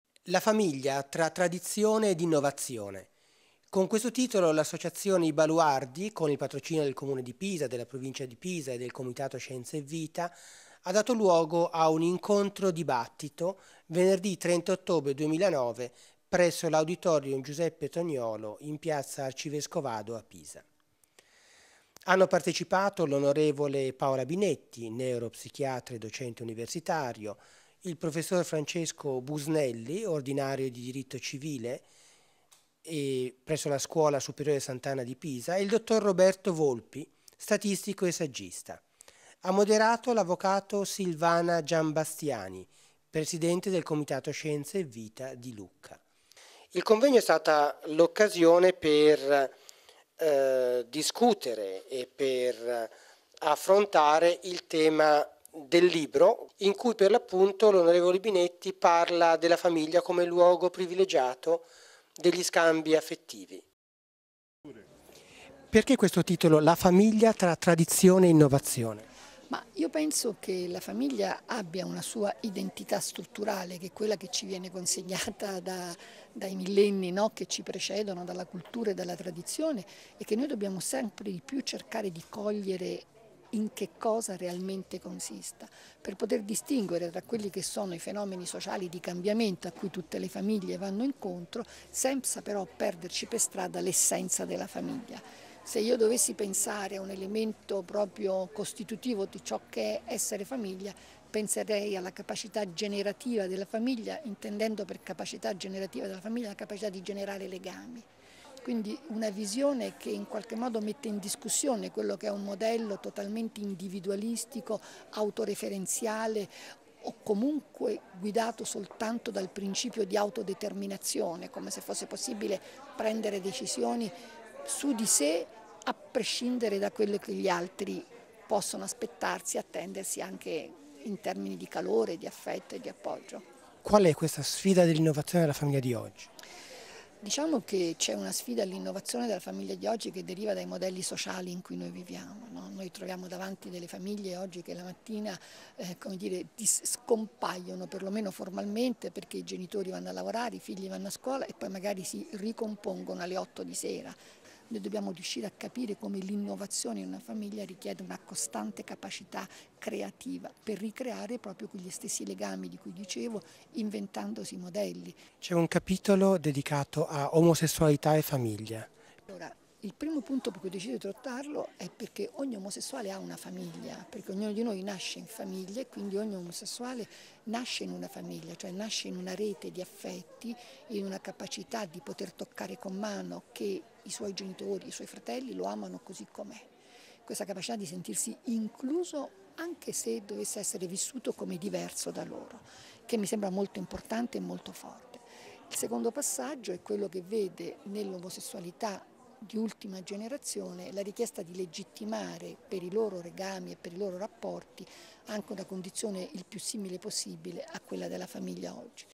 La Famiglia tra Tradizione e Innovazione
Presentazione del libro di Paola Binetti, La Famiglia tra Tradizione e Innovazione, promossa dall'Associazione I Baluardi a Pisa nel novembre del 2009